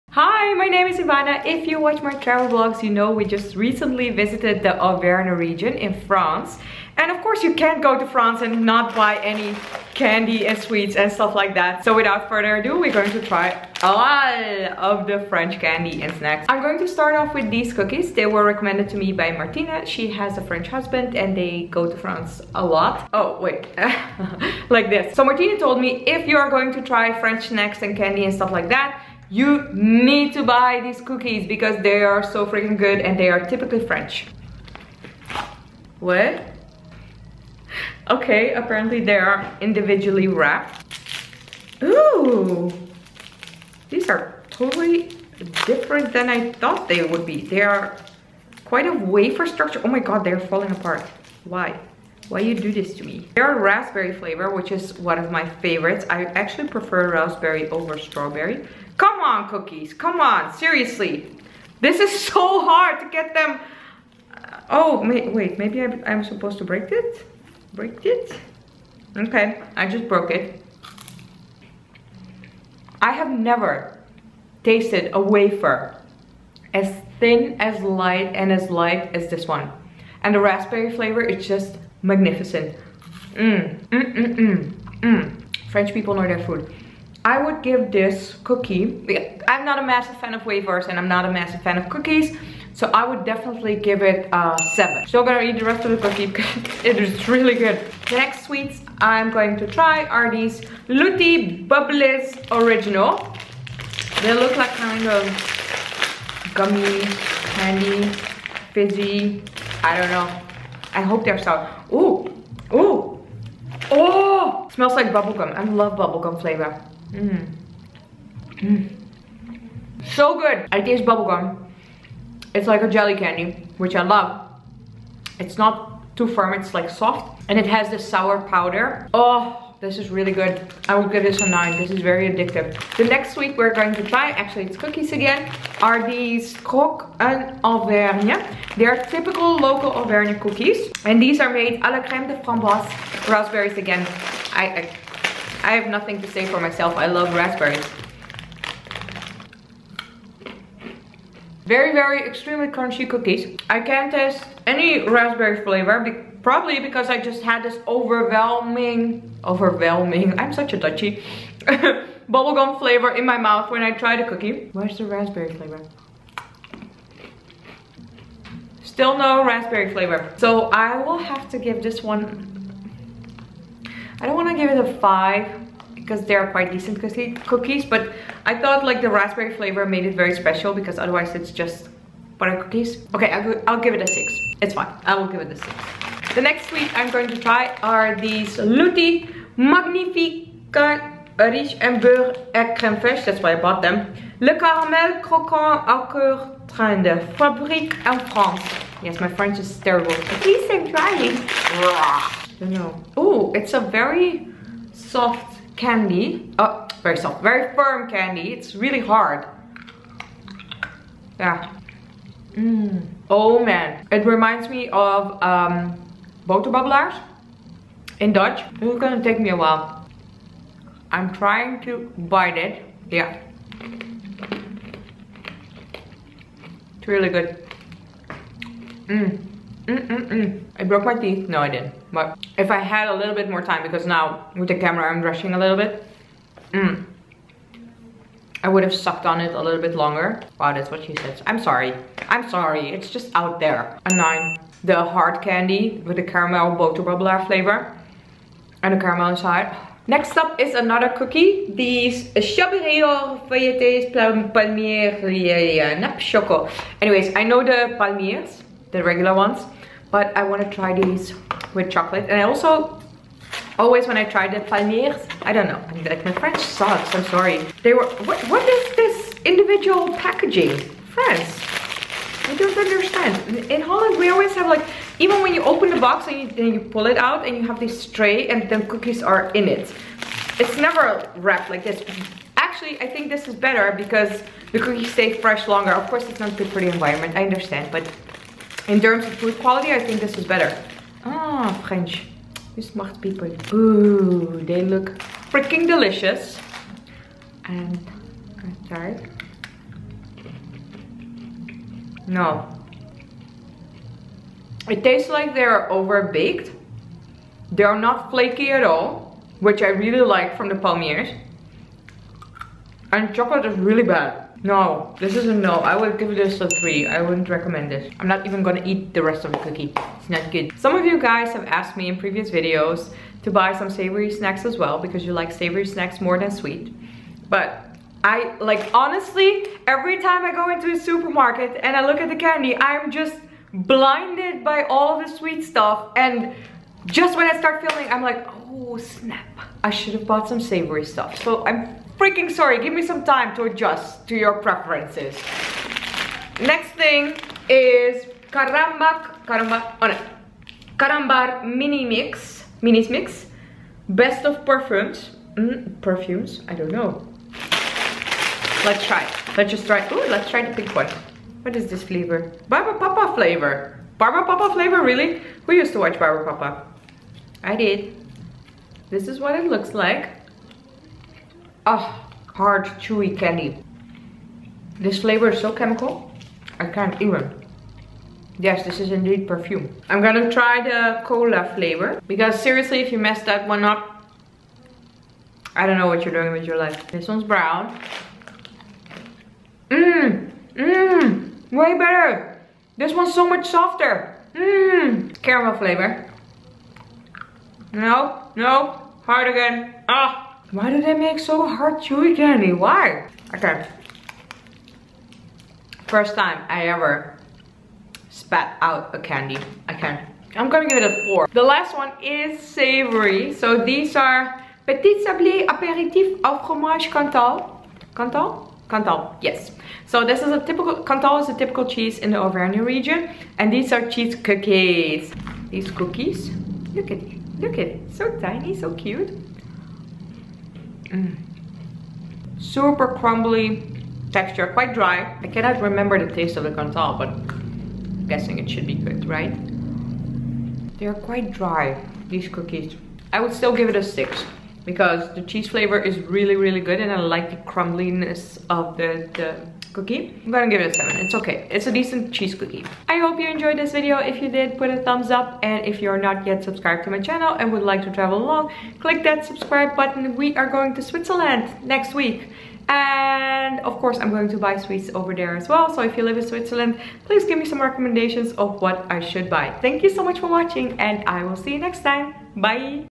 DUTCH GIRL TRIES FRENCH CANDY | FRENCH FOOD PORN | ENTERPRISEME TV
Of course I bought some French candy when we recently visited the French Auvergne region! Totally doing a French candy taste test in this video. Hope you enjoy this French candy tatse test video but
If you're on a roll here's more French food porn in the Auvergne travel vlog: